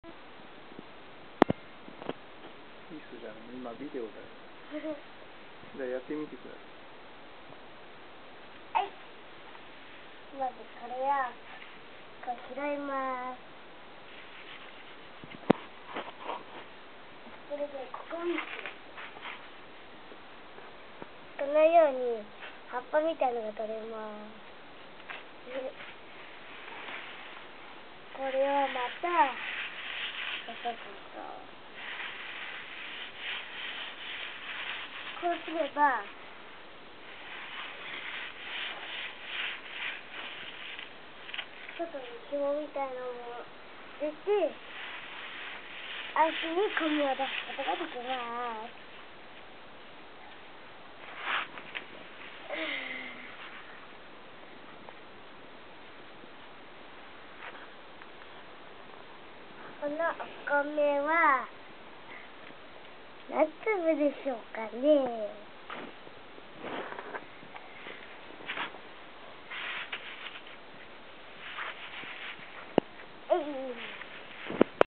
ササニシキのノギ(?)取り
コンバインで収穫後、保存しておいたササニシキの籾。
稲穂から脱粒しただけなので、籾に茎(?)のような物がついている。
これがあると塩水選をする際に軽く無いのに浮いて来てしまうとの事。
そこですりすりして除去する。